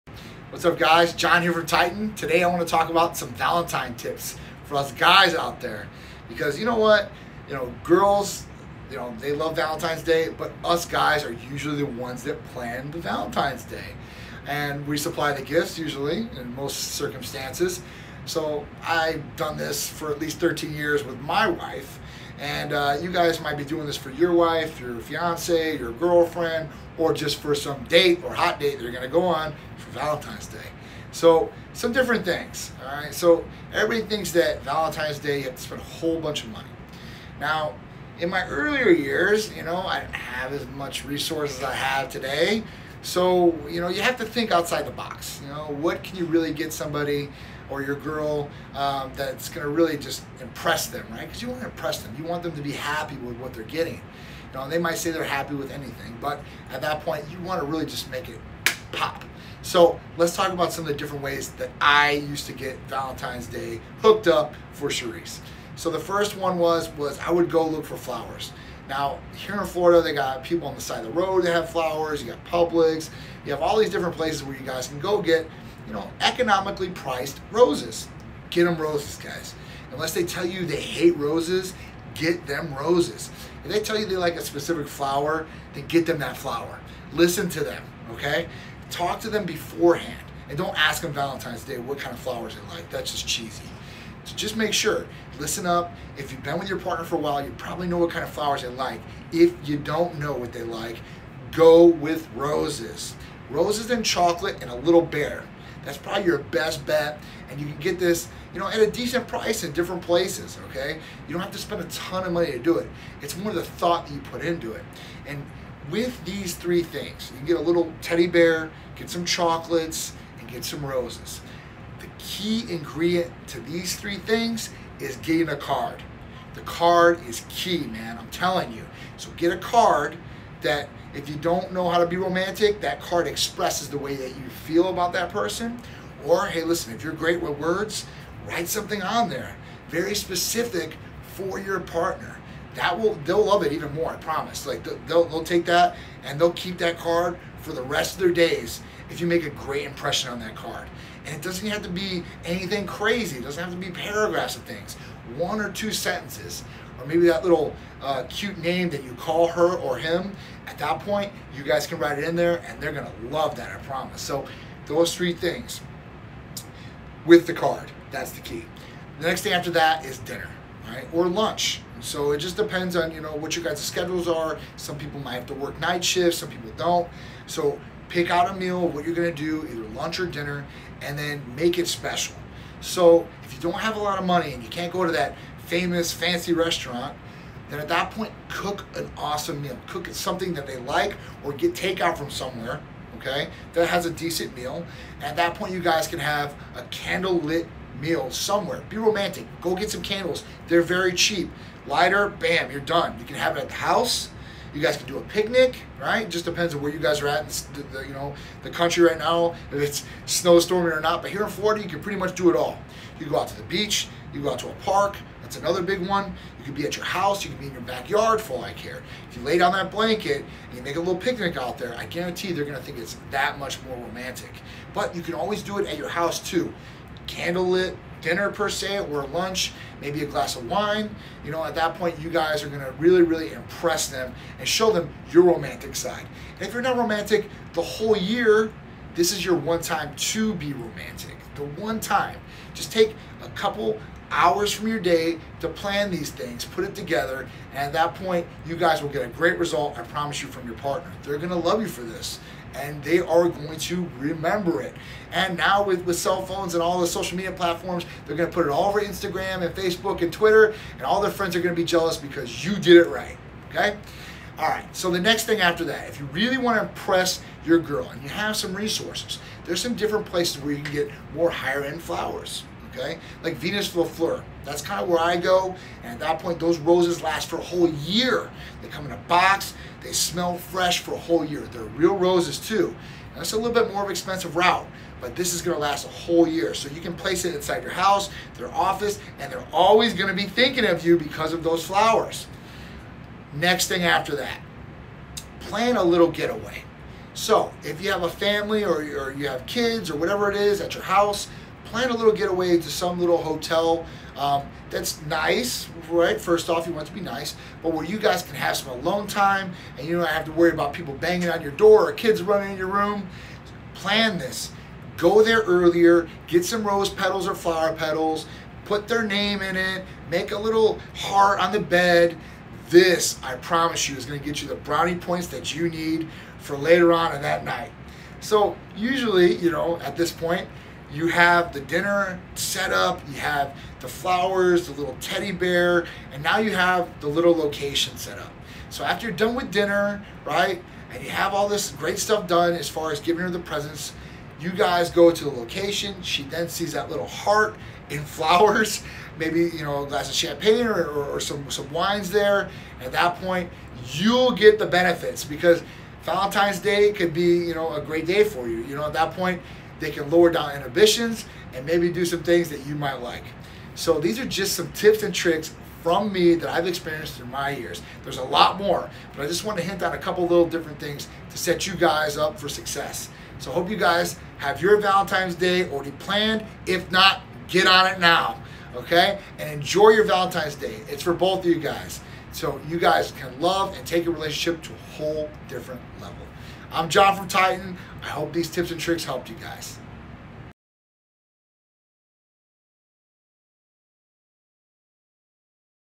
Valentine's Day advice from Titan owner John Tsikouris
Titan owner gives us Valentine's Day advice and how to make it a special!

Or click here for our full list of social media!